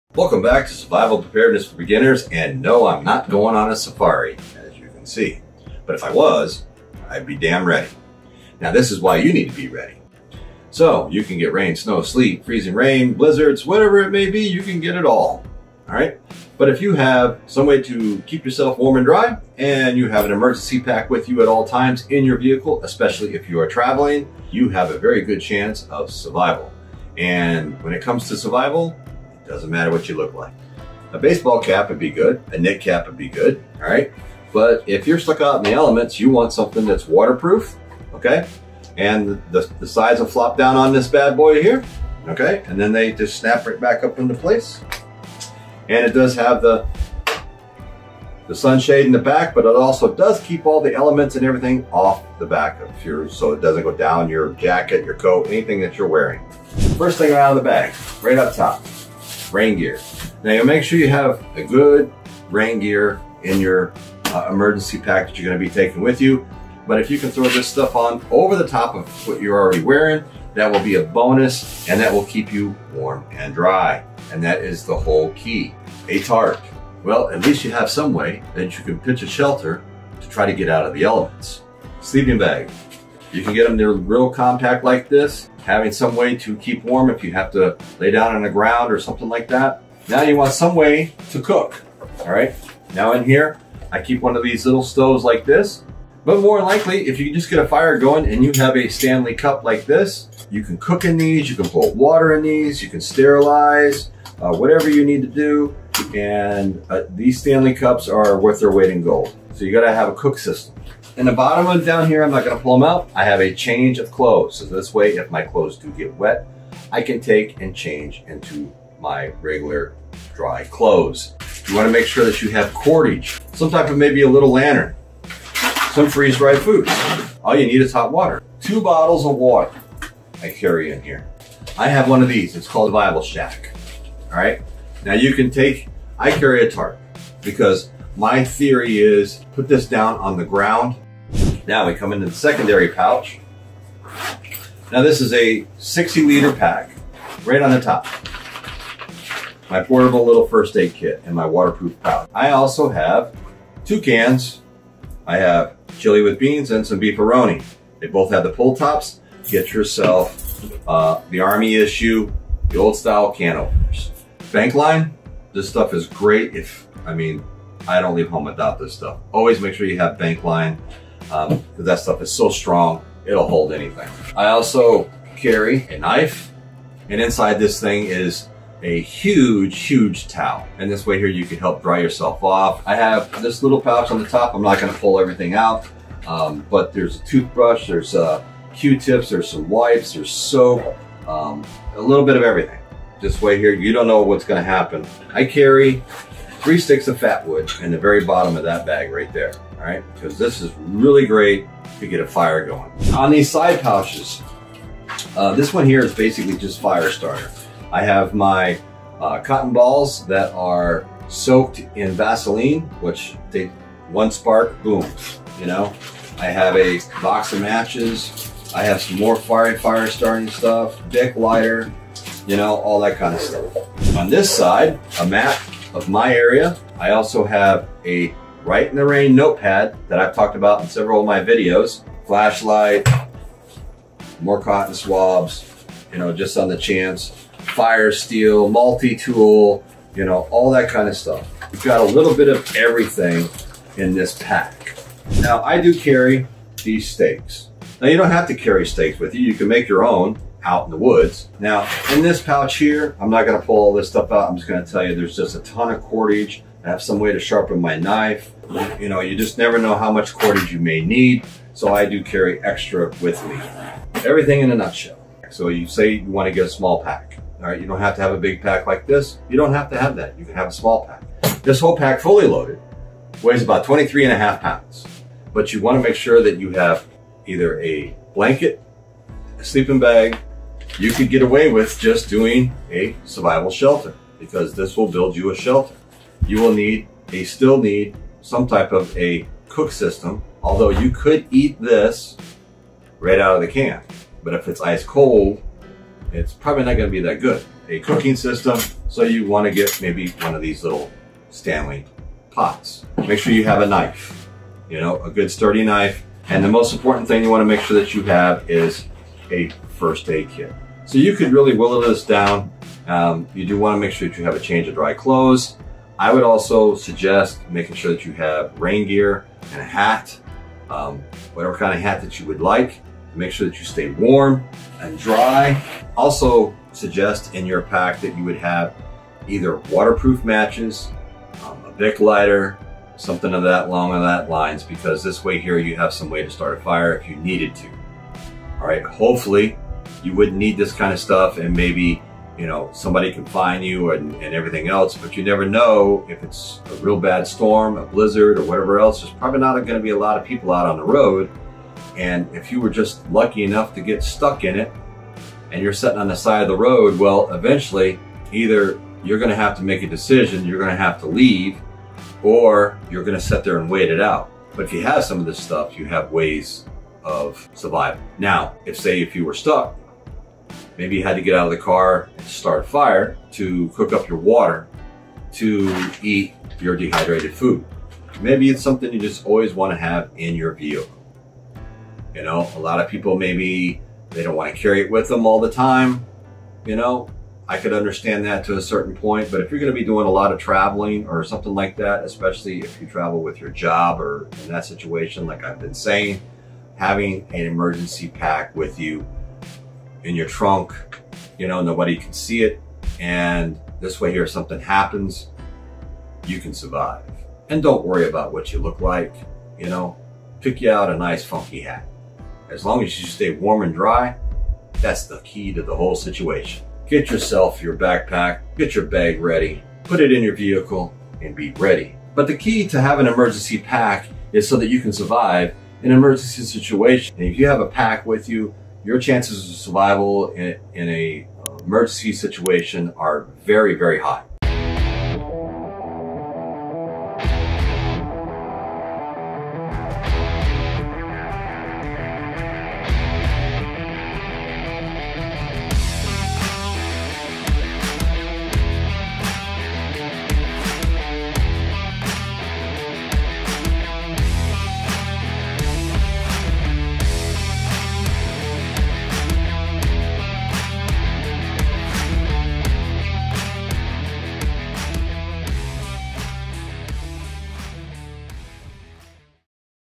Emergency Preparedness Kit:backpack
Building A 72 Hour Emergency Preparedness Kit. This is a remix of a old but great video broken down to the basics. This is so important to have with you at all times.